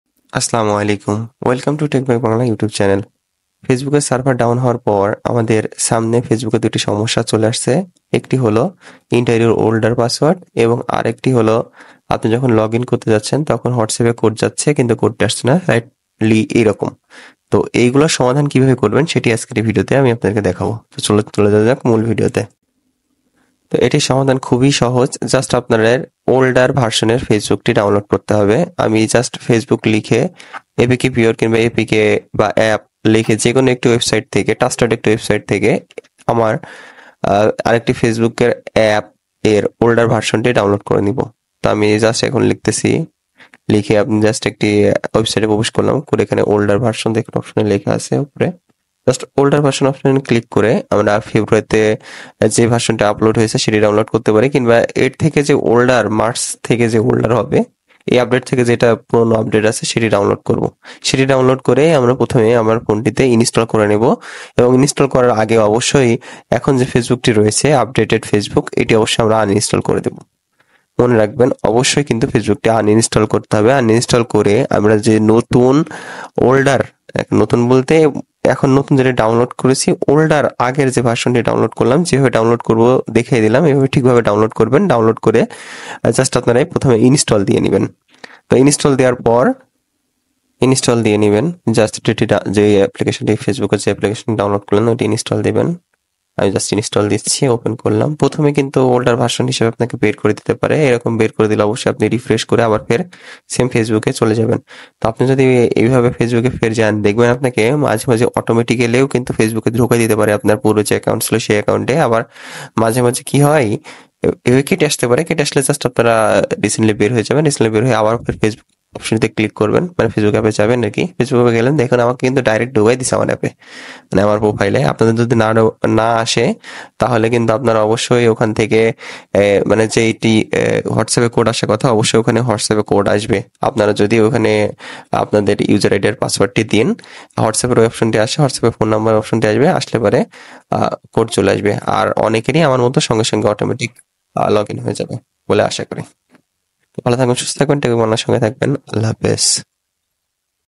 Facebook Update WhatsApp code problem Solved
Our latest update addresses and resolves the issue with WhatsApp code on Facebook, ensuring a seamless and hassle-free experience for users. Update now to enjoy a smoother messaging experience on WhatsApp through Facebook.